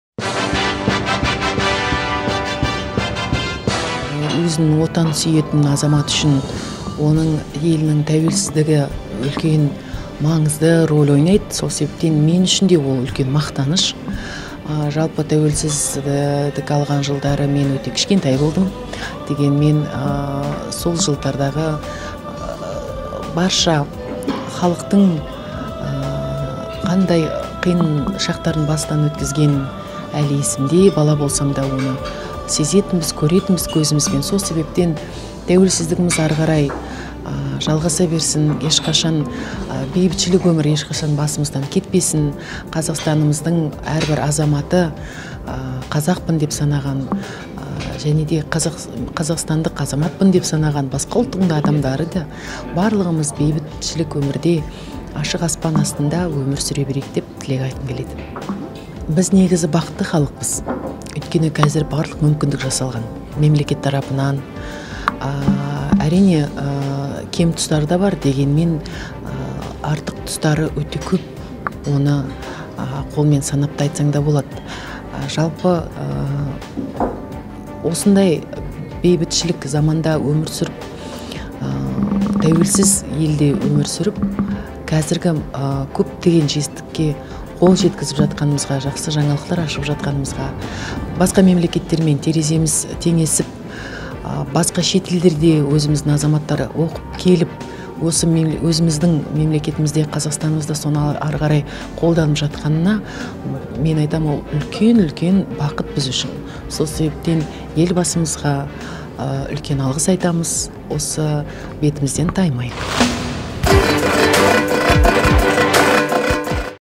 Інжу Әнуарбекқызы
Інжу Әнуарбекқызы Қазақстан халқын "Тәуелсіздік" мерекесімен құттықтады.